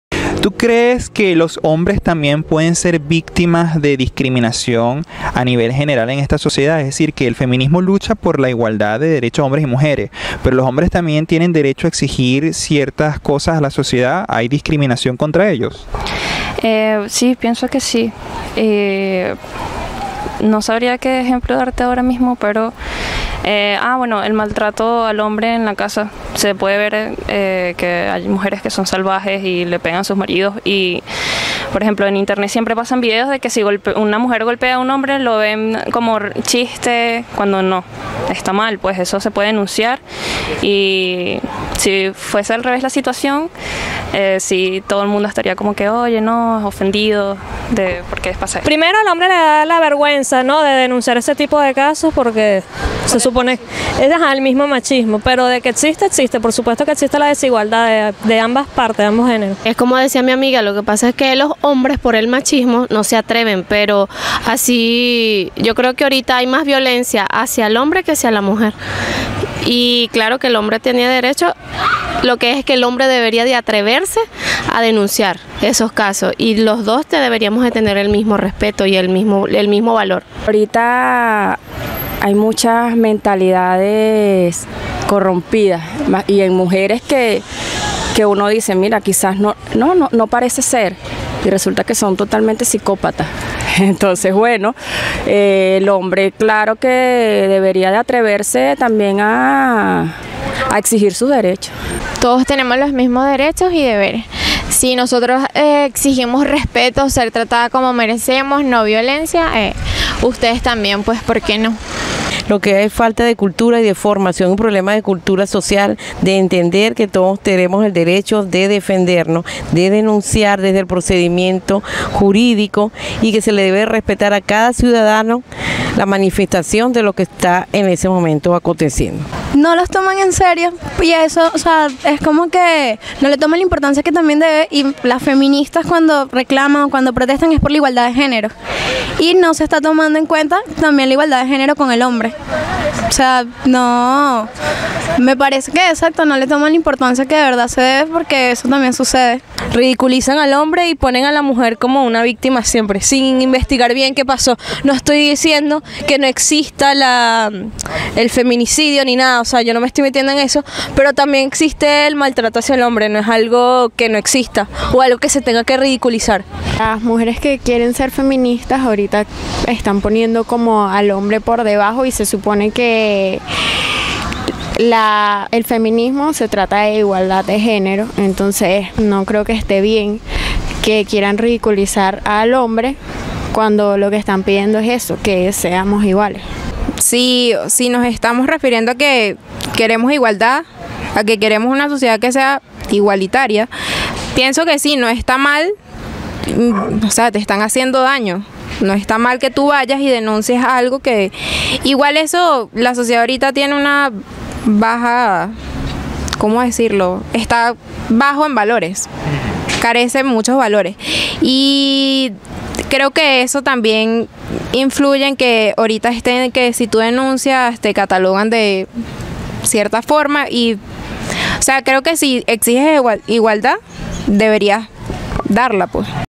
Feminismo en Venezuela: ¿Existe la violencia de género contra el hombre?
Como hoy es el DíaDeLaMujer pensé que sería necesario hablar acerca de la ViolenciaContraElHombre, por eso como parte de las entrevistas que hice a varias venezolanas, les pregunté sobre el Feminismo y sus opiniones de esto, pues este tema es poco tratado y me parece que la gente no le da importancia al mismo.

Por cierto, este vídeo es parte de un proyecto de noticias que estoy haciendo llamado "Persecución Social", por eso su falta de intro, sigue el mismo aquí:

Si quieres donar o aportar algo al canal, puedes hacerlo aquí y muchas gracias de antemano